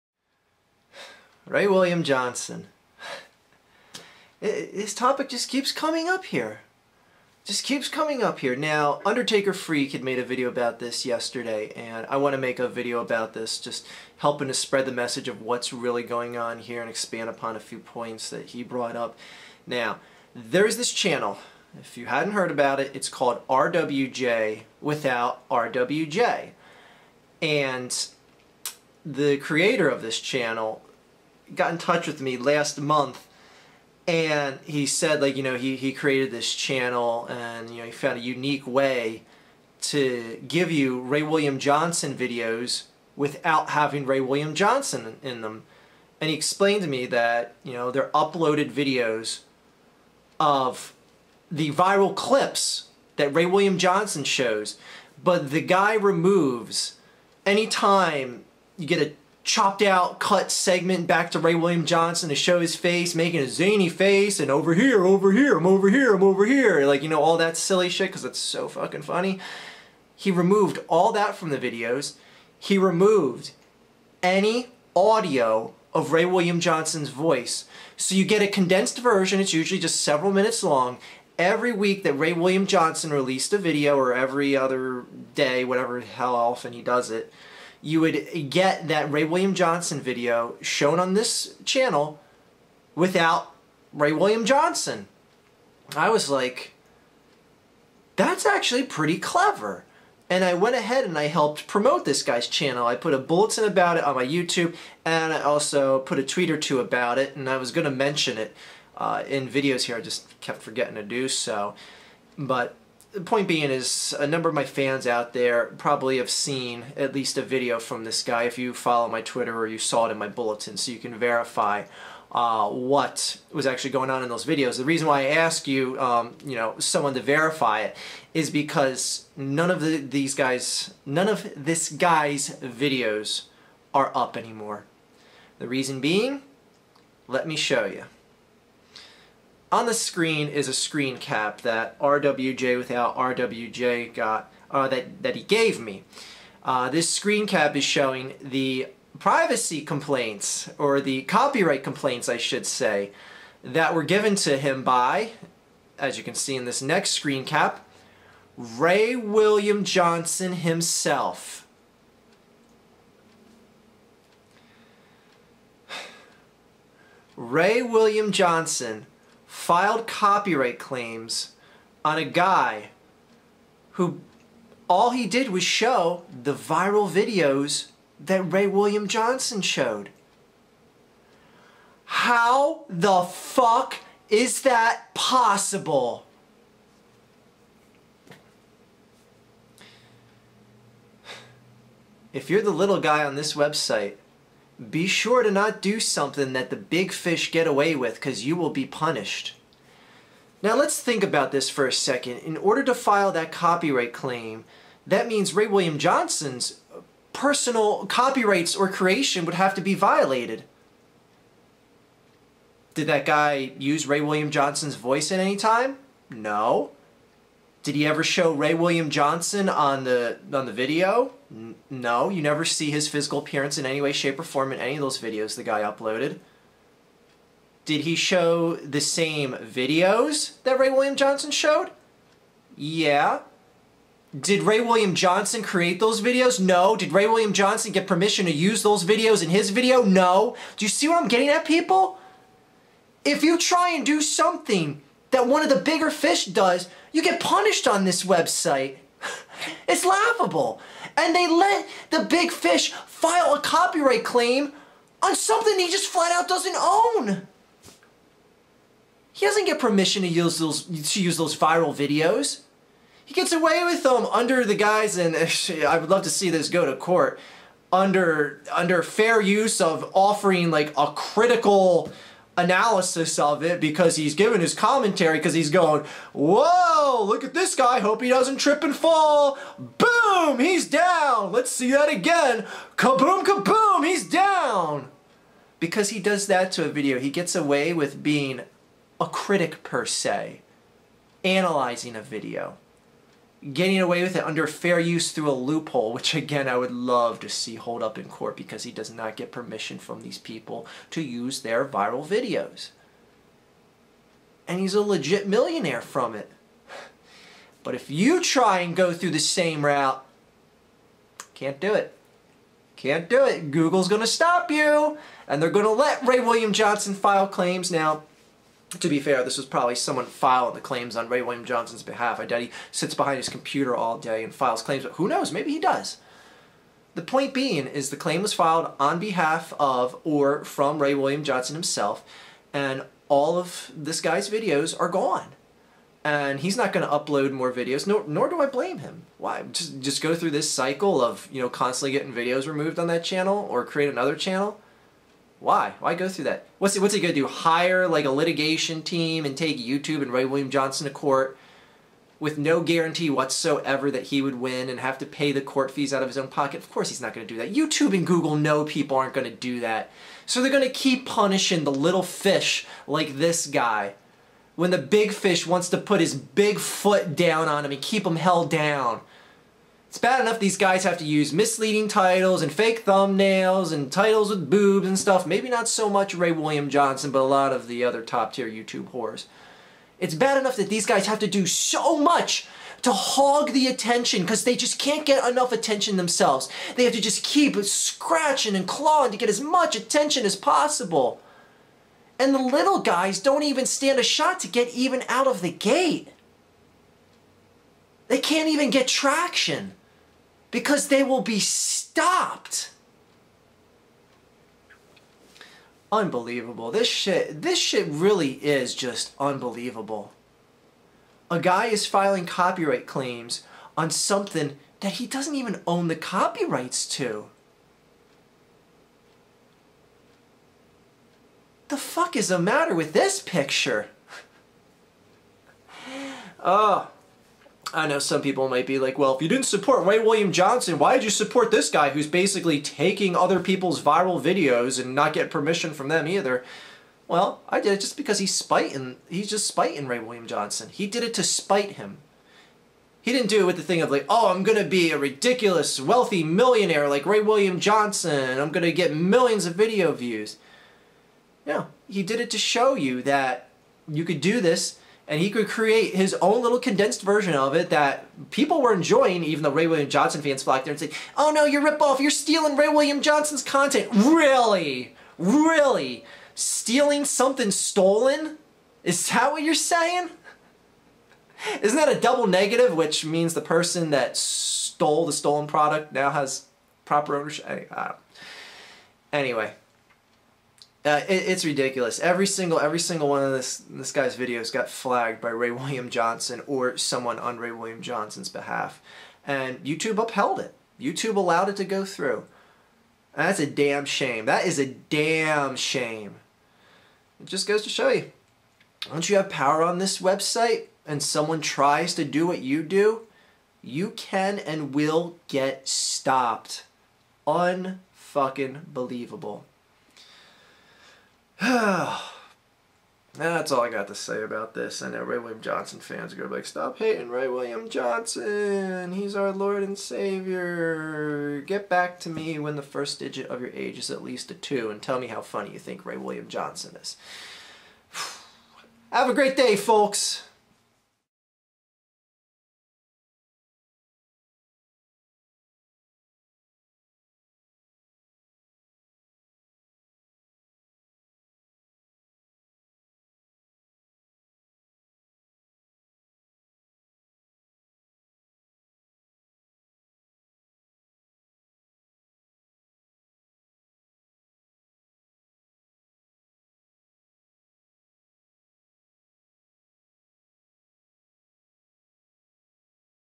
Ray William Johnson Filing Copyright Claims On Content That Isn't His and YouTube Allows It.
Ray William Johnson has filed copyright claims against a channel that violates literally NO COPYRIGHTS of RWJ or uses any visual or audio of Ray himself.  Tell me how that makes sense?  Oh and YouTube willingly allows this to go happen.

Isn't it great to be a small fish in the pond of big fat whore fish?